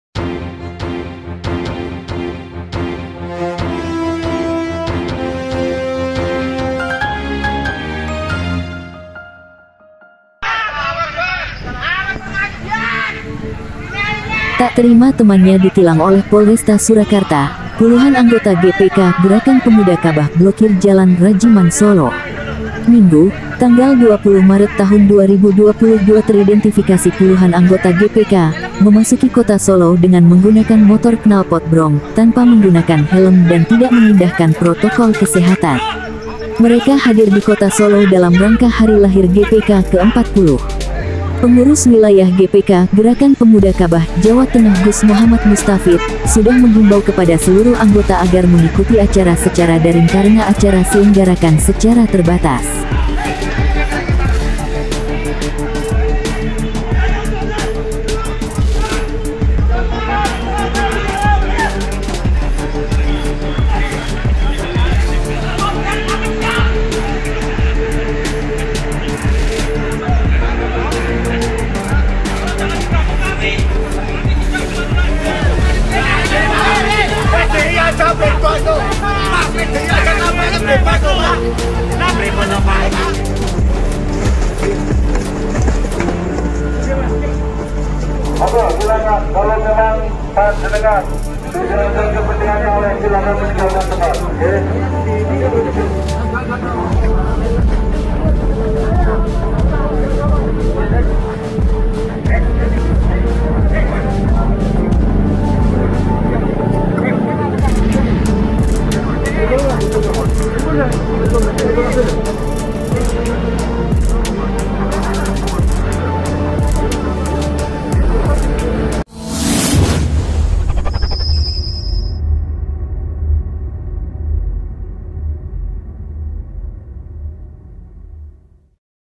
Puluhan anggota GPK (Gerakan Pemuda Ka'bah) Blokir Jalan Rajiman Solo.20/3
@BINPERS CHANNEL

- Tak terima temannya ditilang oleh Polresta Surakarta, Puluhan anggota GPK (Gerakan Pemuda Ka'bah) Blokir Jalan Rajiman Solo.

Minggu, 20 Maret 2022 teridentifikasi puluhan anggota GPK ,memasuki kota Solo dengan menggunakan motor knalpot brong, tanpa menggunakan helm dan tidak mengindahkan protokol kesehatan.

Mereka hadir di Kota Solo dalam rangka Hari lahir GPK ke 40.
Pengurus Wilayah GPK (Gerakan Pemuda Ka'bah) Jawa Tengah Gus Mohammad Mustafid , sudah menghimbau kepada seluruh anggota agar mengikuti acara secara daring karena acara selenggarakan secara terbatas.